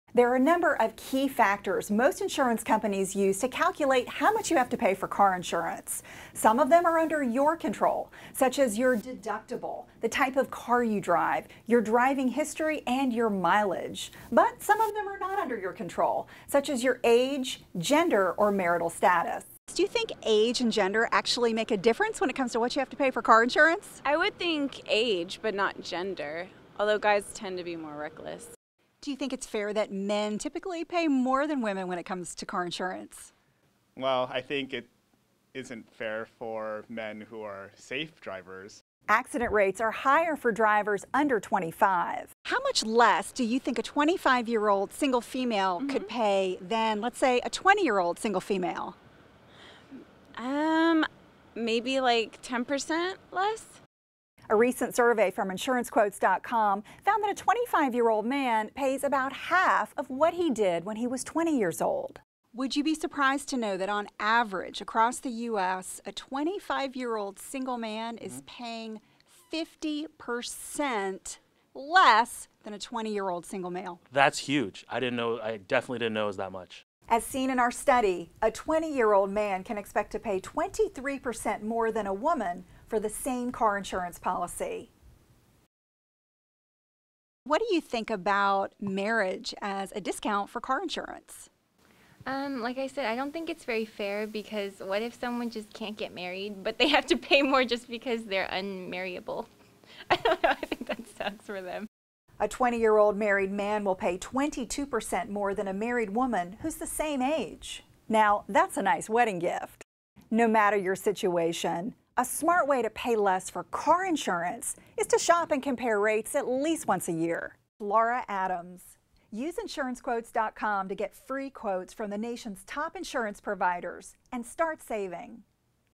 Gender, Age, Marriage and Your Car Insurance Rates
It probably comes as no surprise that car insurance premiums vary between certain demographics. For instance, it's well understood a 20-year-old male driver will most likely pay more for insurance than a man 20 years his senior. What may be shocking, however, is just how significant these premium swings can be -- and age isn't the only factor.

A recent study, commissioned by insuranceQuotes.com, found car insurance premiums, on average, can vary by more than 50 percent based on age, gender and marital status. The study looked at the economic impact of these three demographic designations across the country -- and the results may be startling to some drivers.

To read more about how age and gender, and its affects on your insurance rates click on the